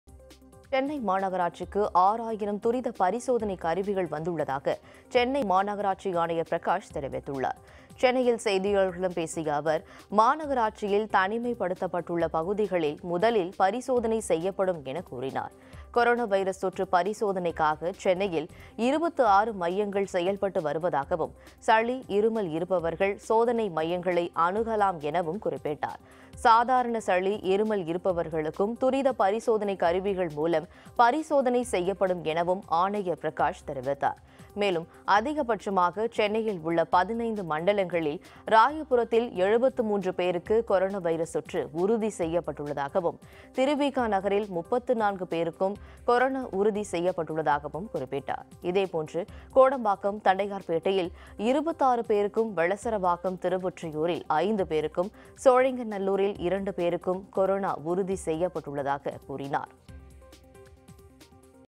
சென்னைக்கு 6,000 துரித பரிசோதனை கருவிகள்
சென்னை மாநகராட்சிக்கு 6 ஆயிரம் துரித பரிசோதனை கருவிகள் வந்துள்ளதாக சென்னை மாநகராட்சி ஆணையர் பிரகாஷ் தெரிவித்துள்ளார்.